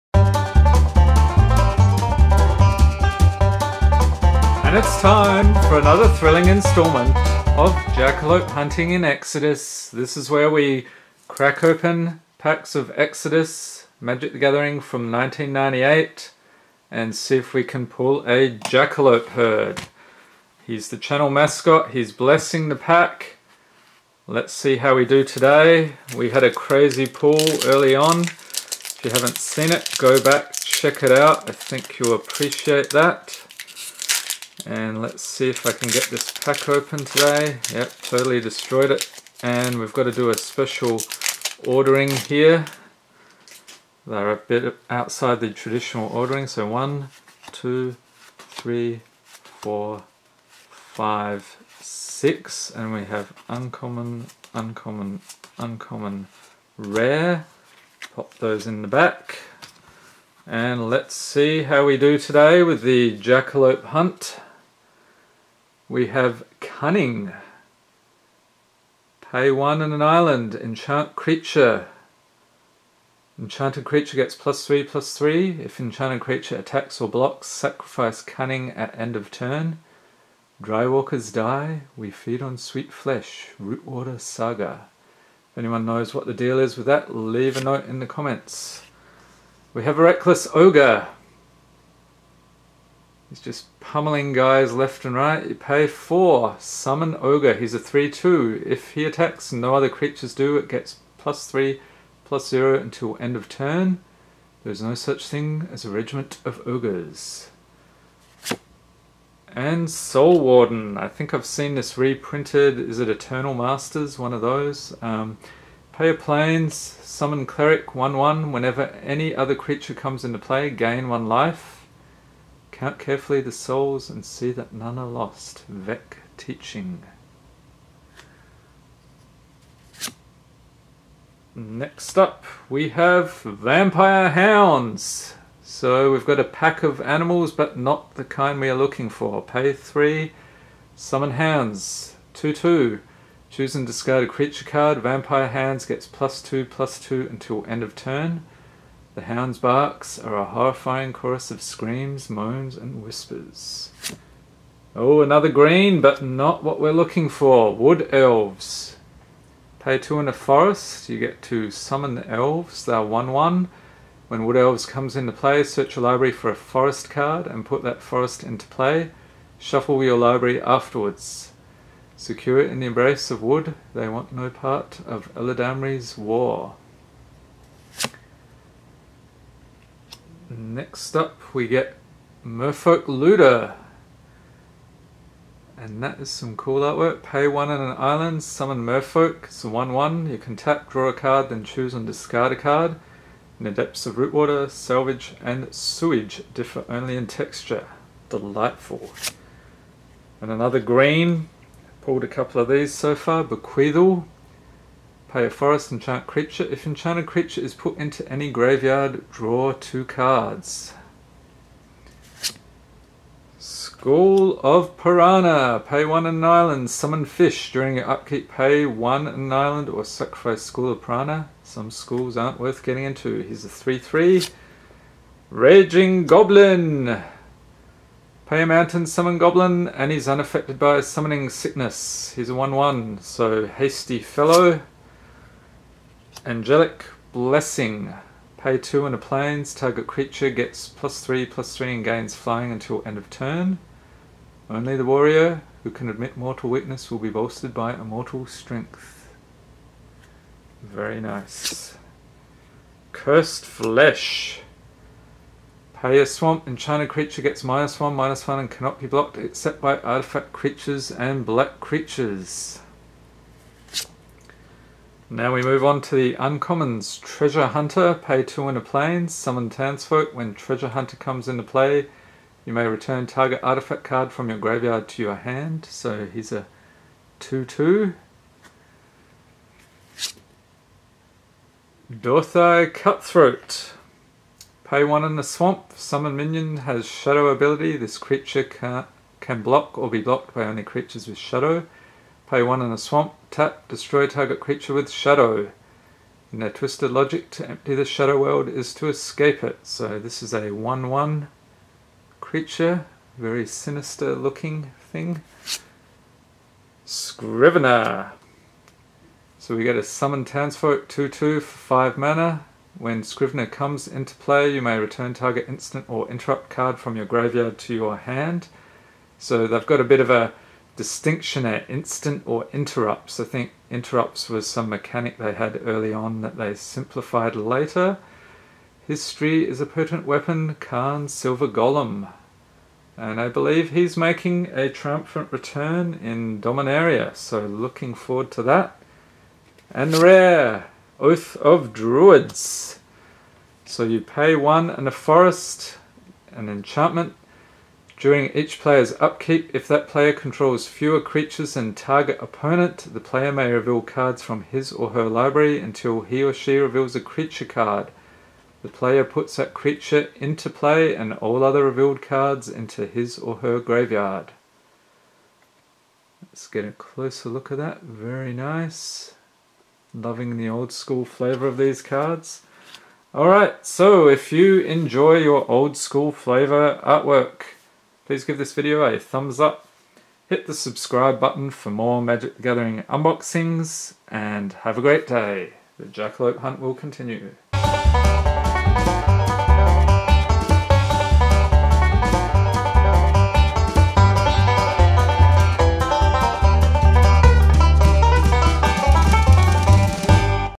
Jackalope Hunting in MTG Exodus #5 pack opening / unboxing
The hunt for the mysterious Jackalope Herd continues with a tasty vintage booster pack from Magic: The Gathering Exodus. This is episode #5 in a bizarre new series!